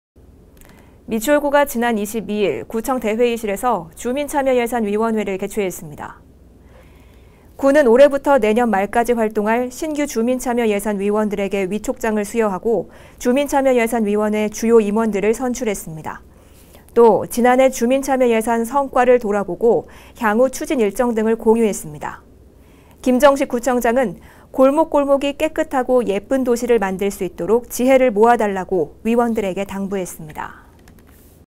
[NIB 뉴스] 미추홀구 올해 주민참여예산위원회 개최
미추홀구가 지난 22일 구청 대회의실에서 주민참여예산위원회를 개최했습니다.

구는 올해부터 내년 말까지 활동할 신규 주민참여 예산 위원들에게
위촉장을 수여하고, 주민참여예산위원회 주요 임원들을 선출했습니다.

또 지난해 주민참여예산 성과를 돌아보고 향후 추진일정 등을 공유했습니다.

김정식 구청장은 ‘골목골목이 깨끗하고 예쁜 도시를 만들 수 있도록 지혜를 모아 달라.’고 위원들에게 당부했습니다.